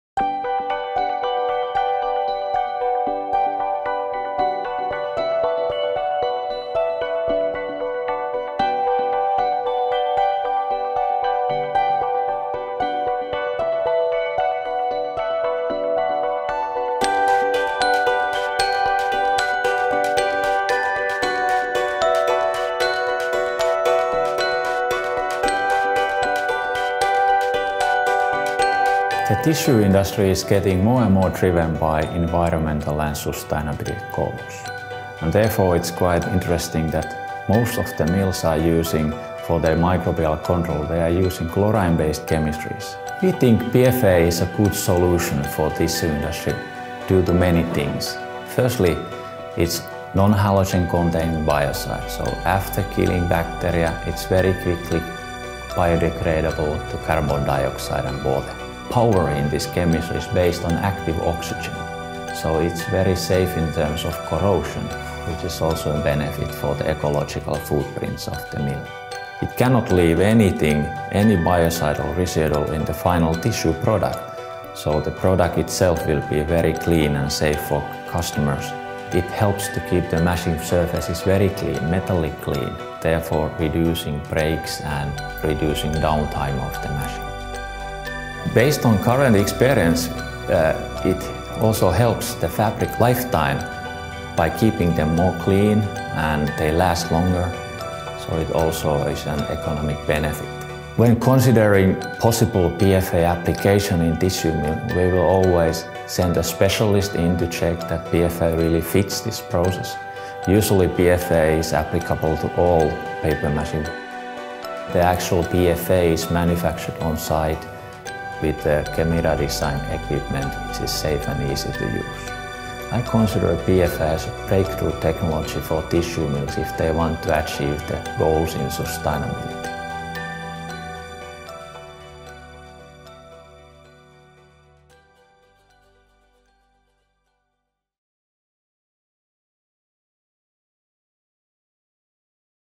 Kemira Fennosan PFA: revolutionizing microbiological control in tissue mills
PFA is a great solution for the tissue industry: it's a non-halogen containing biocide. It biodegrades to carbon dioxide and water very quickly after killing bacteria.

The tissue industry is driven by environmental and sustainability goals. This technology has the potential to revolutionize microbiological control in tissue mills.

Fennosan PFA is now available in the EMEA area.